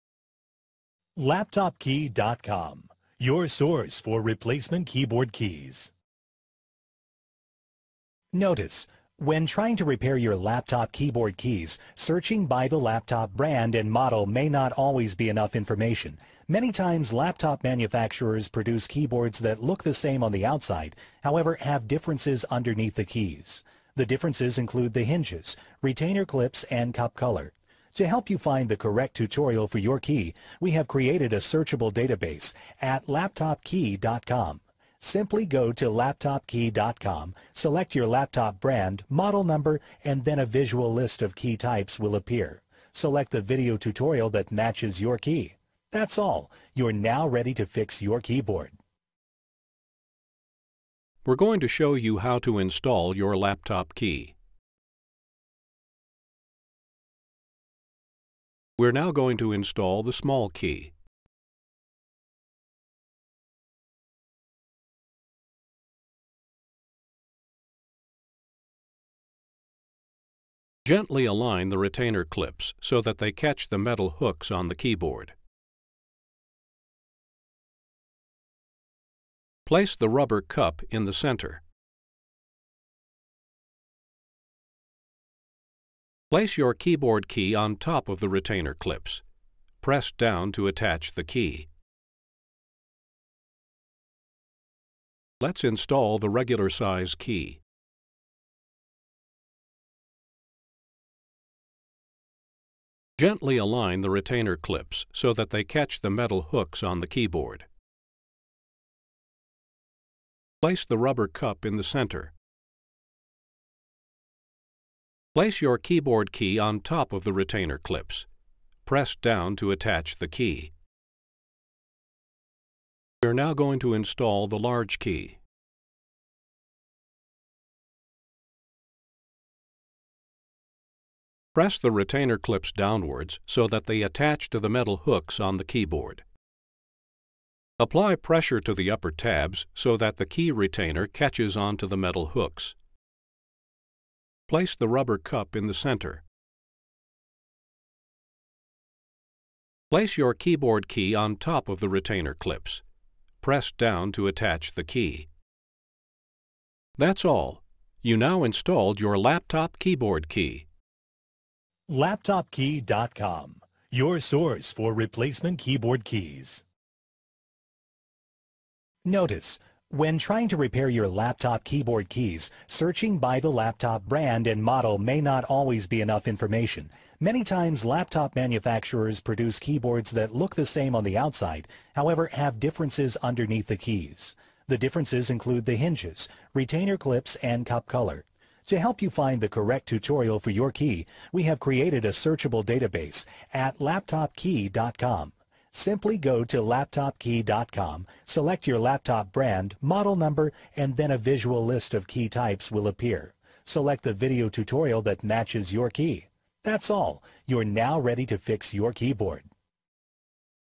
Netbook Laptop Keyboard Key Repair | Fix Install Stuck Sticky Keys | HP ENVY M6
Single Laptop Keyboard Keys Repair Guide for HP ENVY M6
M6-N010DX (Silver)
M6-N (Silver)
6037B0100201 (Silver)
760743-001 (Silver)
M6-N012DX (Silver)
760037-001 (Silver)
M6-N000 (Silver)
97-00076-US-0B-00R02 (Silver)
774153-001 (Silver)
6070B0766502 (Silver)